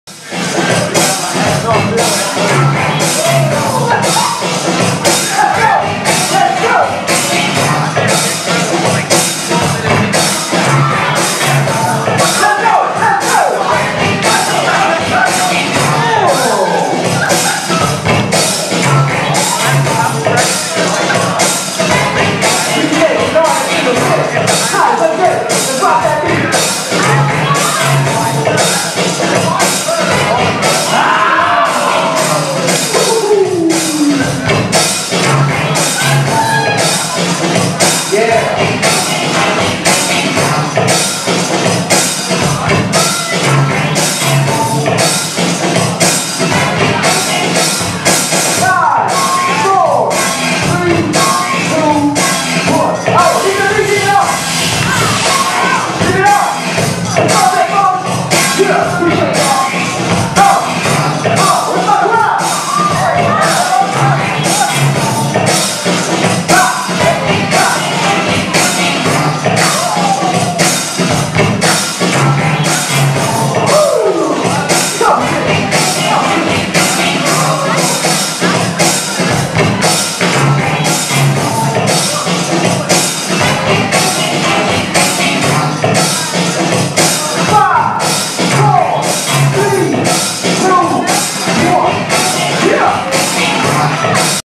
Beat Geeks v J Fung & Nancy, Tiebreaker 1
YA Battles on Thursday, September 3, 2015 at The Island, St. Martin's Episcopal Church in Houston, Texas with DJ Canarus